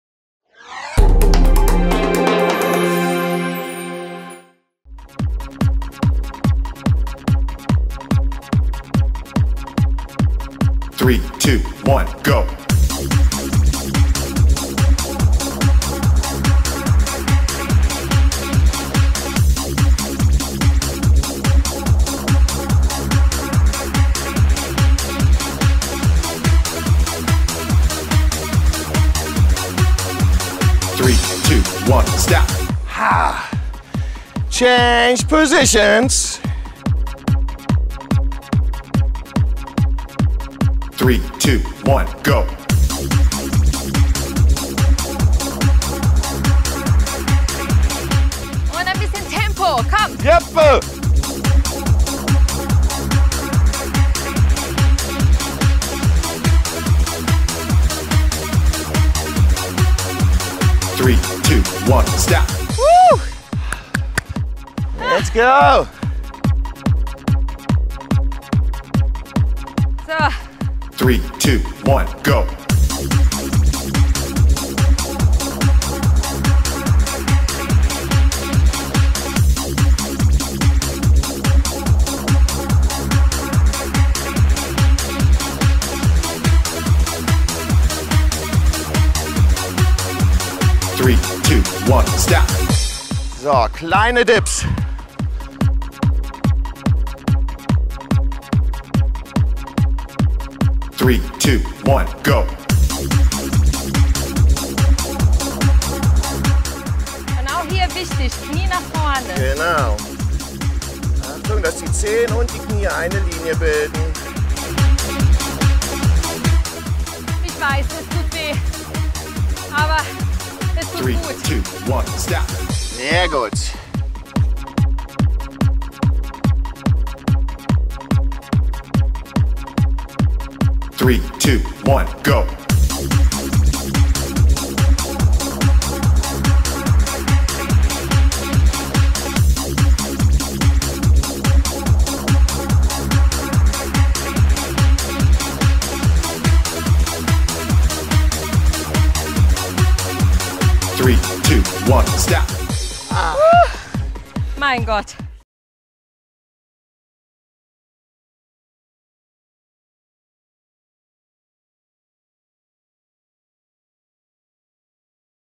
Zum knackigen Beachbody in 6 Wochen - Workout (2) II FitnessFriends
Die jeweils 10-minütigen Workouts und einzelnen Fitness-Übungen verhelfen dir schnell und effektiv zu einem straffen und wohlgeformten Beach Body. So wirst du dich in deinem Bikini oder Badeanzug noch wohler fühlen.

➤ Altersfreigabe: FSK Info-Programm

Das erfolgreiche Model Jana Ina Zarrella und Fitness-Experte Detlef Soost präsentieren vier funktionelle Ganzkörper-Workouts und bringen deinen Bauch, deine Beine und deinen Po in Strandform.

Ob knapper Bikini, raffinierter Monokini, sportlicher Tankini oder ganz klassischer Badeanzug – beim Thema Bademode hast du stets die Qual der Wahl. Die Frage „Und welcher Bikini passt zu mir?“ stellt sich da oft. Deshalb verrät dir Model Jana Ina Zarrella zusätzlich, welche Bikinis und Badeanzüge am besten zu jeder Figur passen.